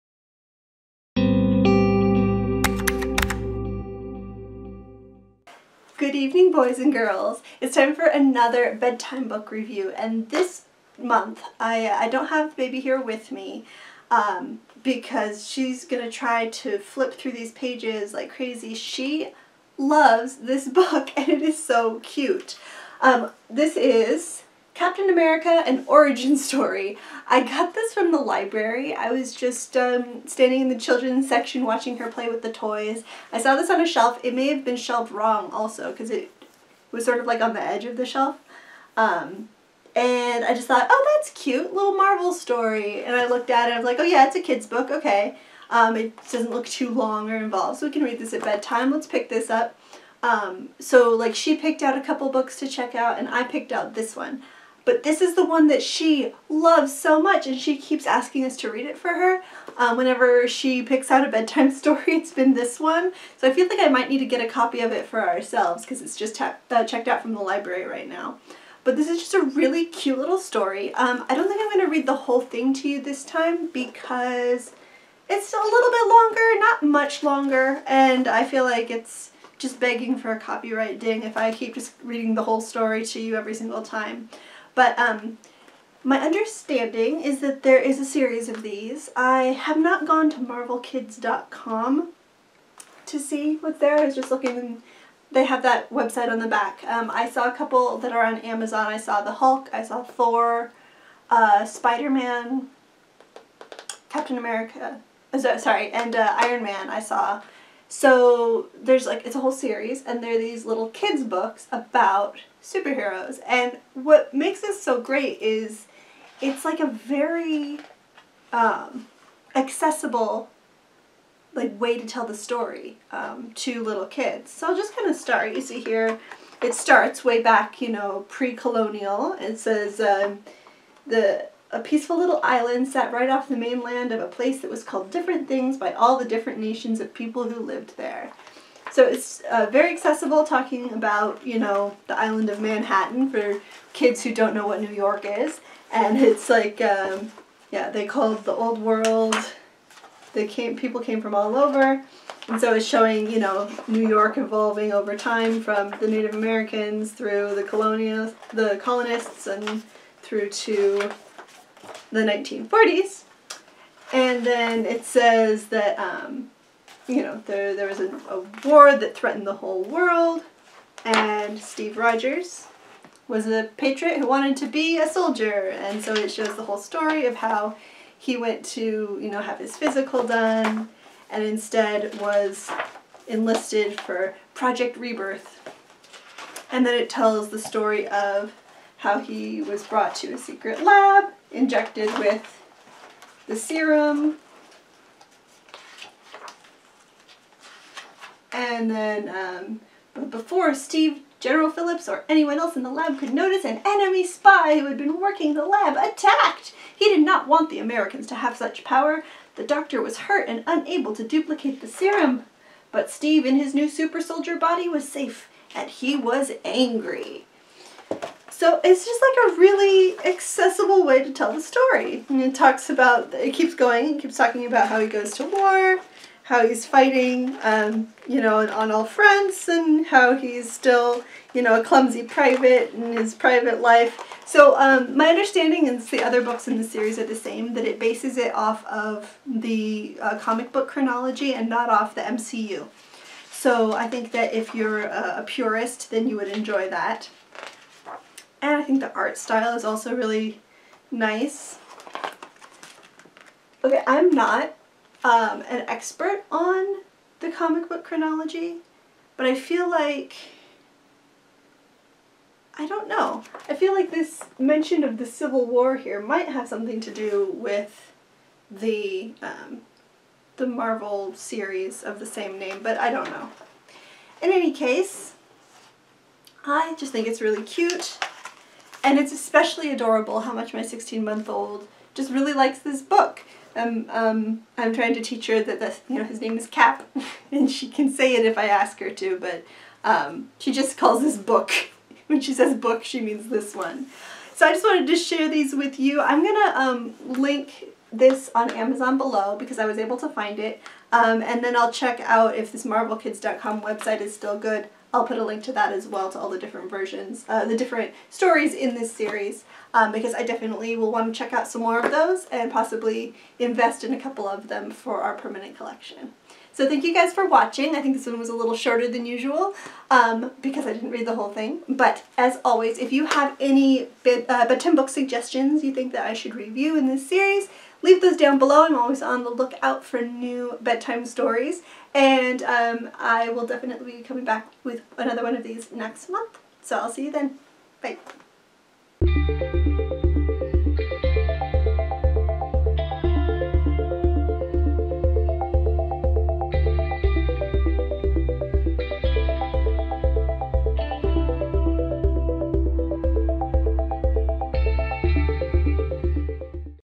My Toddler LOVES this Captain America Book
I picked this book out at the library, and Agnes has been loving it!
I looked at marvelkids.com and it does not seem to list the rest of the series.